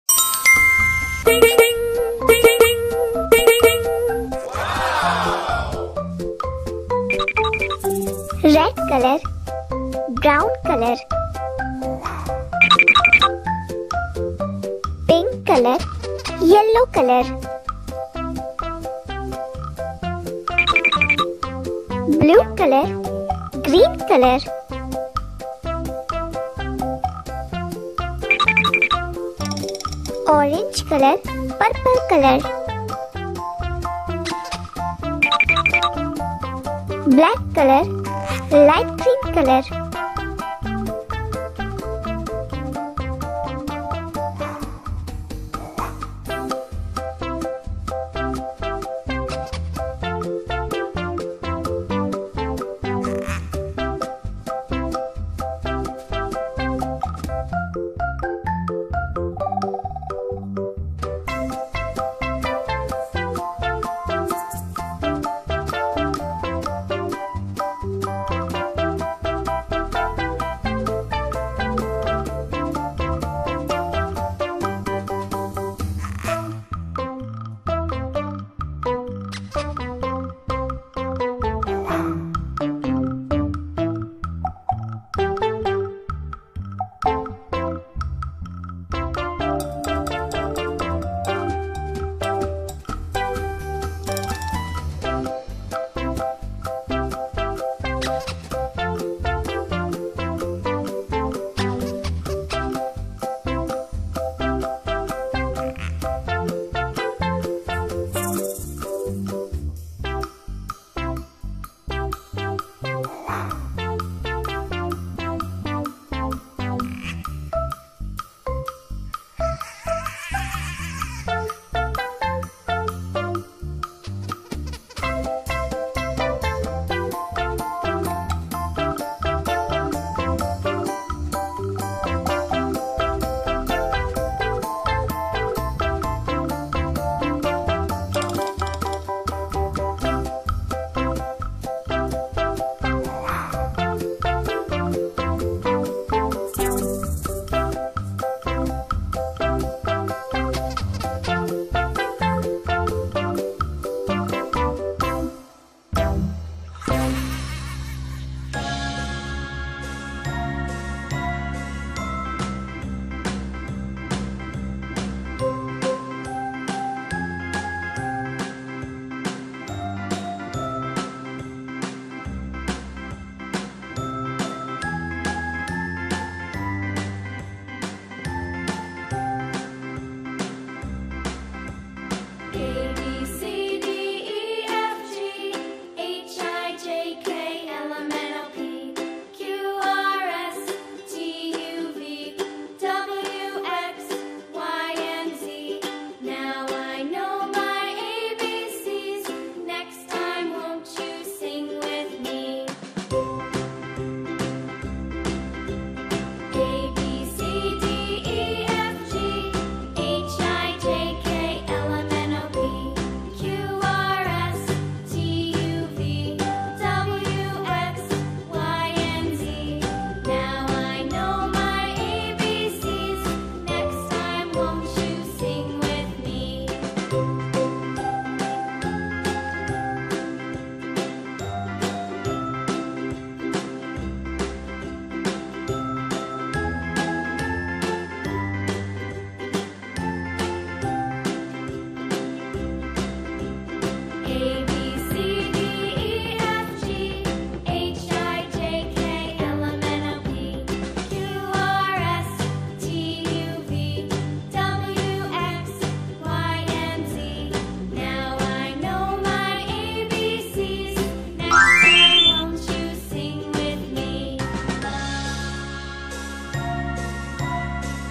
How to draw an ice-cream| easy ice-cream drawing for kids and Toddlers, bacho ki drawing
hii kids let's learn how to draw an ice-cream .first we will learn how to draw ice-cream with the help of marker then we will learn how to do rainbow colouring in our beautiful ice-creams